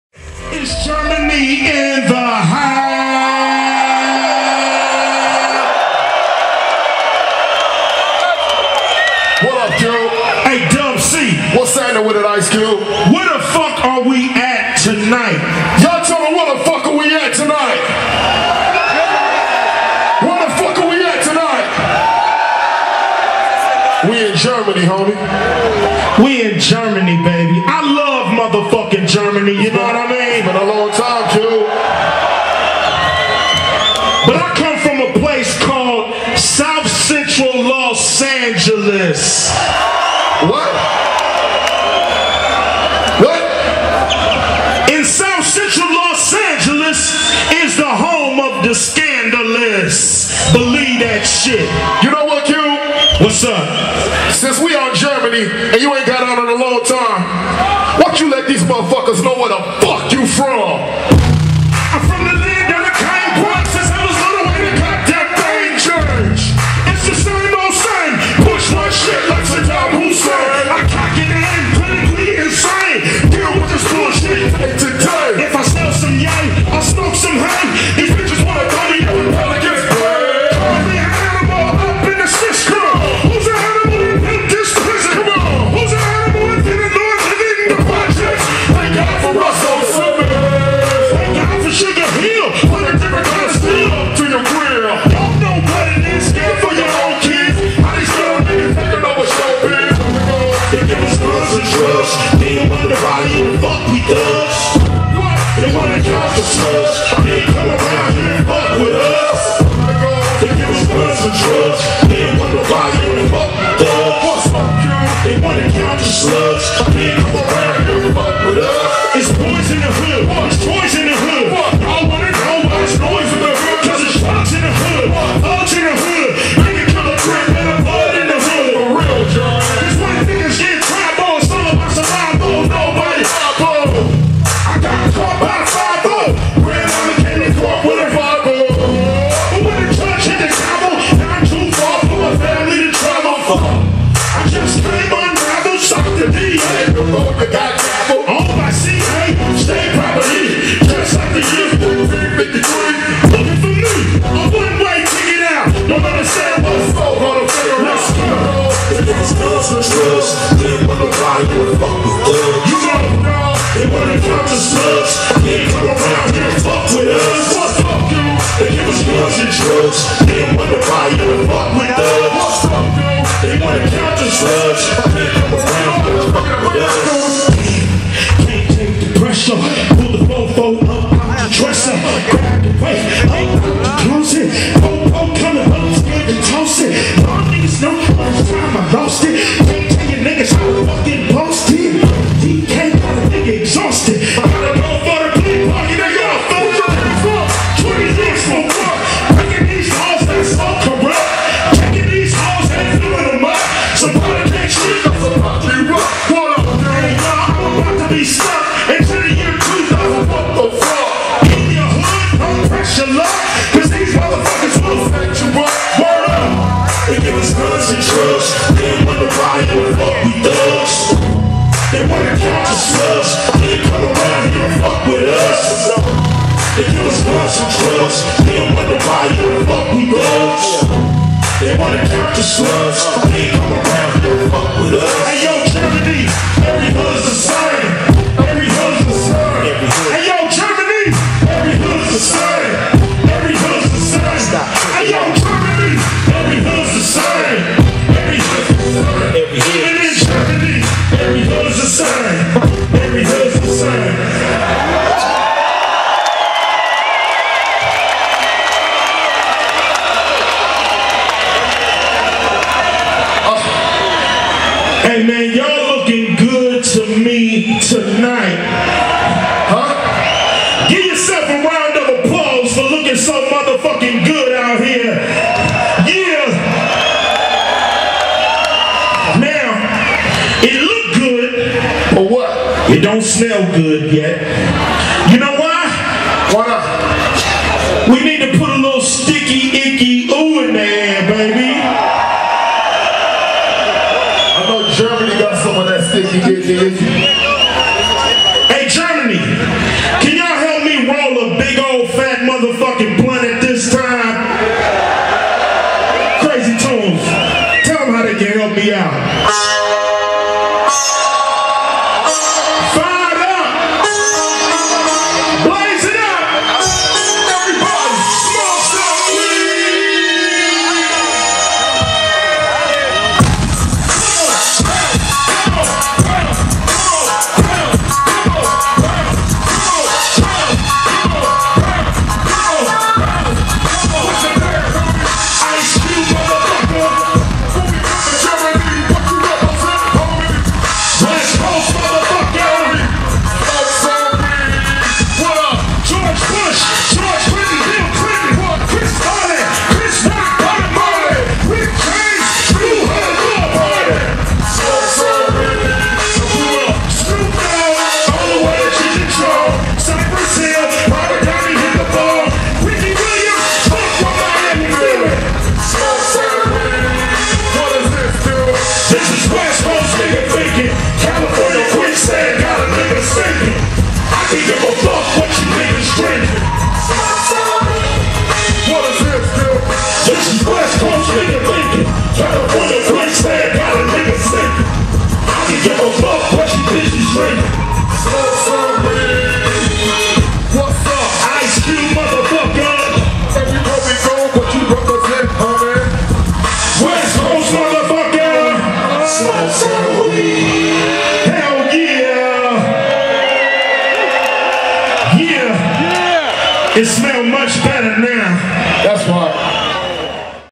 ICE CUBE SPLASH 2008 Part 2
ICE CUBE LIVE AUF DEM SPLASH 2008 AUF DER HALBINSEL POUCH BEI LEIPZIG